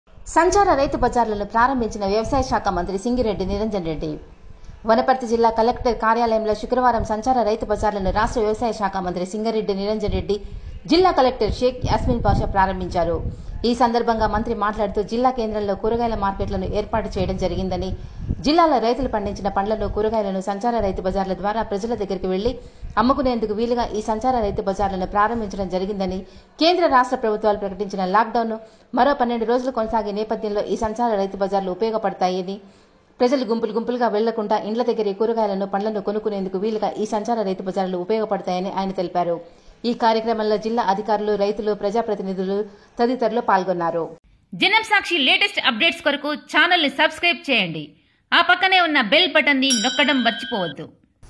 సంచార రైతు బజార్ల ను ప్రారంభించిన వ్యవసాయ శాఖ మంత్రి సింగిరెడ్డి నిరంజన్ రెడ్డి
CORONA VIRUS